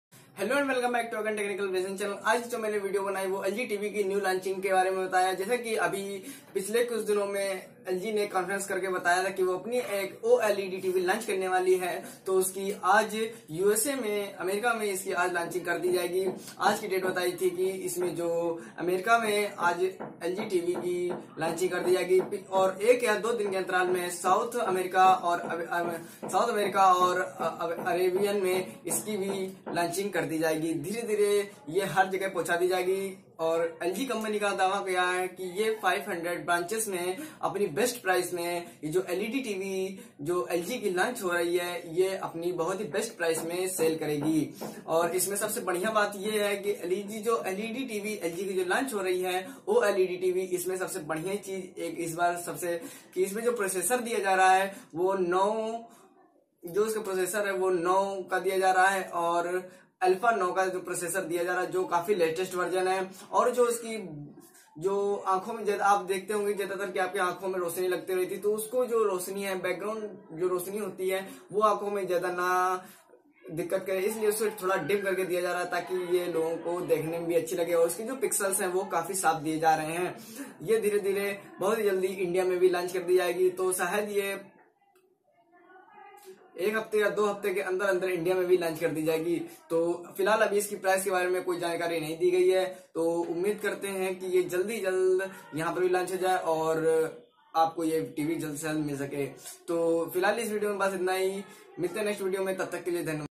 LG OLED TV- Dolby Cinematic Experience
LG OLED TV powered by Active HDR with Dolby Vision and Dolby Atmos, gives you a complete Cinematic Experience. The Self-lighting pixel reproduces perfect black which, in turn, creates perfect colors. The Black makes all the colors more detailed and stunning, as it lends the perfect intensity to each color by enhancing the hidden detail of every color, making it all the more stunning. With world-renowned vision and sound quality, it promises to up your entertainment quotient a thousand times, giving you the best Tv-viewing experience.